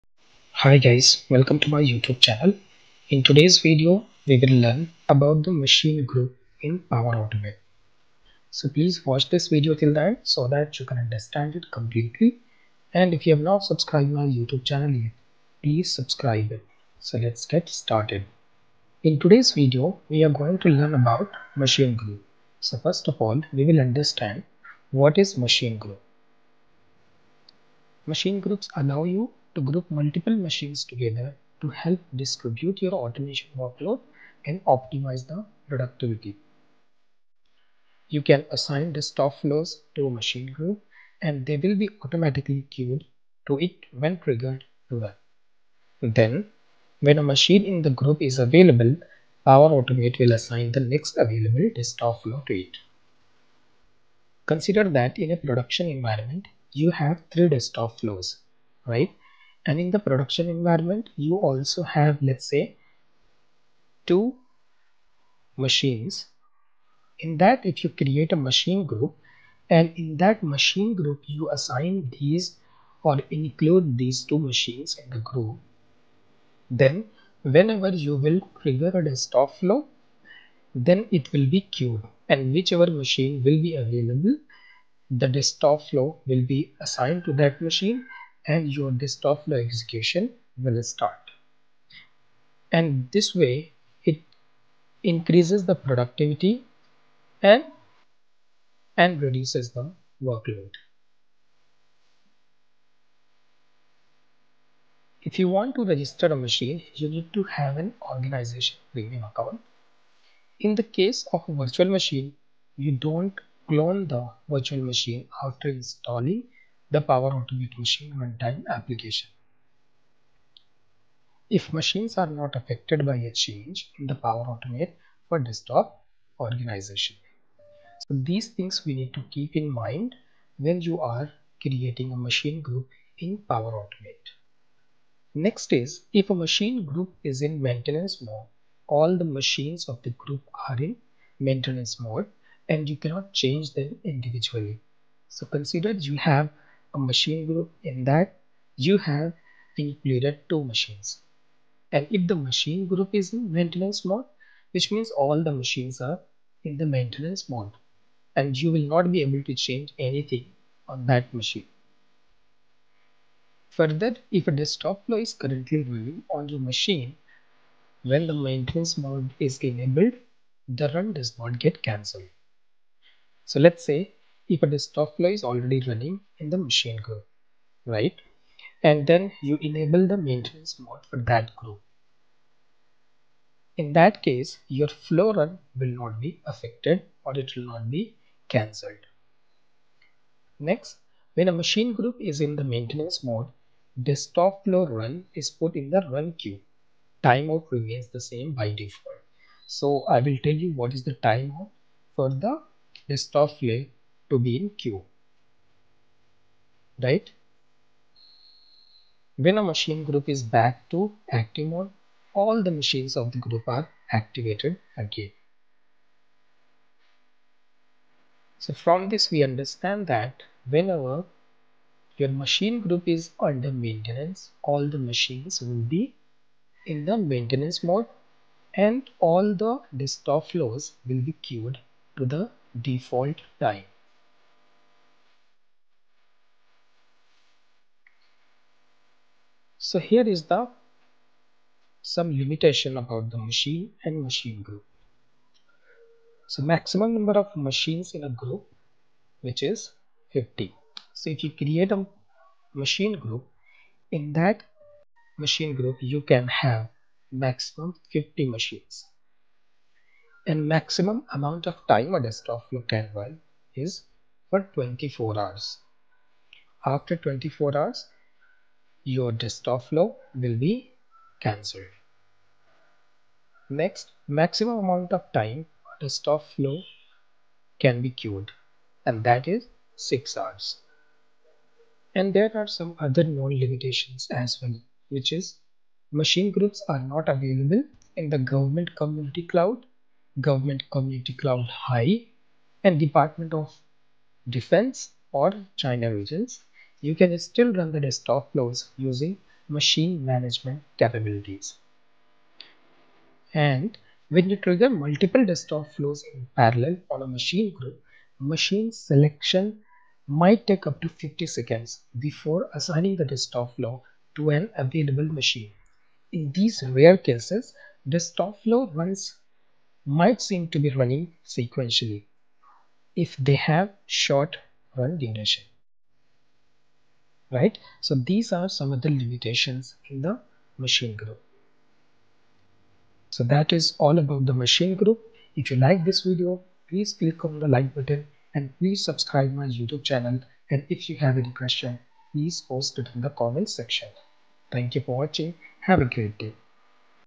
Machine Groups In Power Automate
In this Video, you will learn about machine group in Power Automate. Please watch this video till the end to understand it complete.